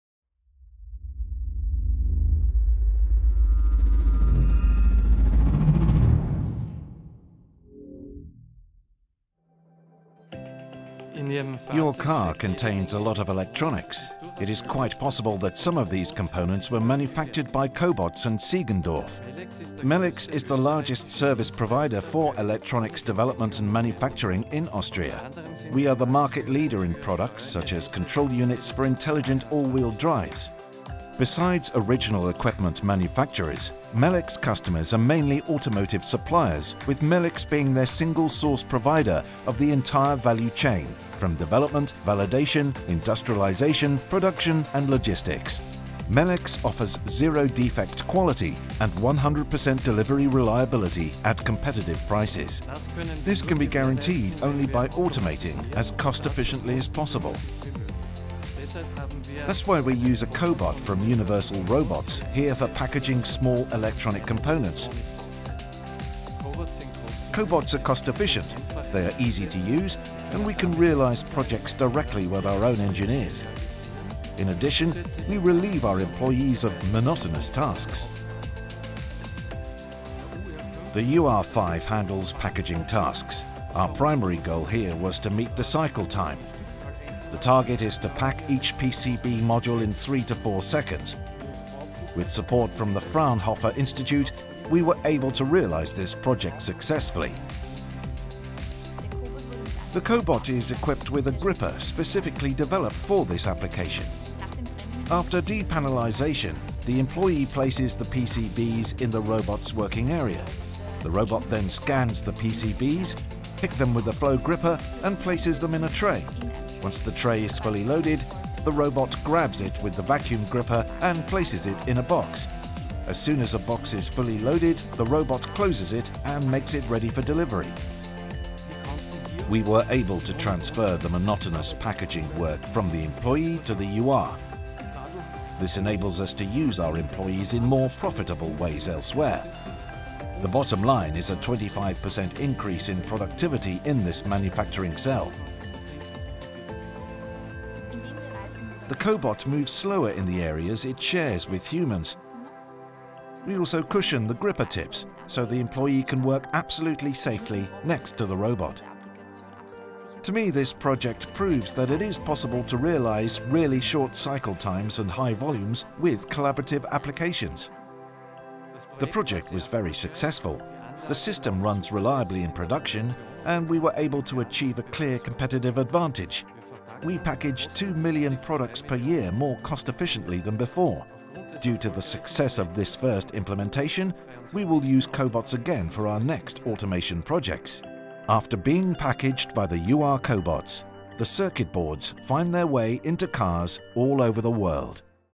【ユニバーサルロボット】Melecs EWS（オーストリア、電子機器）の協働ロボット導入事例：マシンテンディング
UR5で回路基板を年間200万枚パッキング
生産性が25％向上

概要:
　電子機器メーカーのMelecs EWS社（以下、Melecs）はURの協働ロボットを導入し、年間で200万枚の回路基板（PCB）の梱包工程に利用。経費削減にも成功しました。特殊設計のグリッパを搭載したUR5が、一度に3つのPCBをハンドリングするなど、複数作業を一度に実行します。その結果、この工程での生産性が25%アップしました。

課題:
　短期間で成長することを目指したオーストリア企業のMelecsは、従来のソリューションよりもコストパフォーマンスの高い、スピーディな自動化の方法を模索していました。Melecsはイノベーションプロジェクトを実施して、協働ロボットが自動化にどの程度役立つのか評価したいと考えました。

　MelecsはまずFraunhofer Austria Research社と共同で、作業員を反復作業から解放するためのパイロット事業を立ち上げることにし、それに適した工程を探しました。協働ロボットで自動化する最初の作業には、送水ポンプ用電子部品の梱包が選ばれました。それまで、円形の小さなPCBを取り扱う単純作業は3シフトの手作業で行われていました。

　この用途の条件はなかなか厳しいものでした。このシステム開発を担当したFraunhofer Austria Research社のロボティクスエンジニア Titanilla Komenda氏はこう語ります。「最大の課題は、モジュールひとつあたりの梱包に3〜4秒というかなり短いサイクルタイムでした。これを安定して行うことを目的に、ユニバーサルロボットのUR5を選びました。URロボットはコストパフォーマンスが抜群です。それに、すでに市場での実績があり、Melecsの近くにパートナー企業もあるのでサービスがすぐに受けられます」

ソリューション:
　この用途で求められる厳しい条件を満たすには、URロボットはさまざまな作業をこなす必要があります。そのため、ロボットにはカスタムメイドのグリッパを取り付けました。1台のロボットにレーザースキャナ3台、フローグリッパ3個、真空グリッパ1個を搭載しています。

　パネルからPCBを取り外すと、作業員は1セットのPCBをUR5の作業エリアに配置します。つぎにUR5が、まずレーザースキャナで基板を3枚スキャンします。それから3個のフローグリッパが基板を1枚ずつ持って、3枚を一度にトレイに配置します。トレイには基板が54枚置けるようになっています。トレイがいっぱいになったら、UR5が真空グリッパで箱に入れます。箱がトレイでいっぱいになると、UR5が真空グリッパでふたを閉じます。これで箱が発送できる状態になりました。

　Komenda氏は誇らしげに語ります。「URロボットはPCBを年間約200万枚、エラーなしで箱詰めしてくれます。このプロジェクトで、URロボットは非常に短いサイクルタイムを実現できることがわかりました」 Georg Loisel氏も全面的に満足しているといいます。「ユニバーサルロボットを使うようになって、この工程の生産性が25％向上しました。ロボットは3シフト制のもと、とても安定して稼働してくれます。投資は18カ月で回収できる見込みです。URロボットを導入して大正解でした。今後のプロジェクトにもぜひ採用しようと思います」